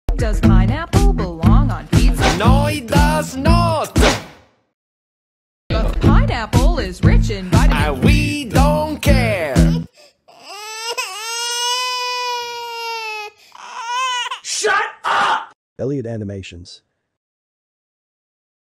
Does pineapple belong on pizza? Meme recreation
Pineapple pizza is sinful!
Plus, this is also the first video that features Childish Kitty.

Created: August 21st, 2023
Originally Edited on iPad Air 4 (2020)
Posted to YouTube on the M2 iPad Pro (2022)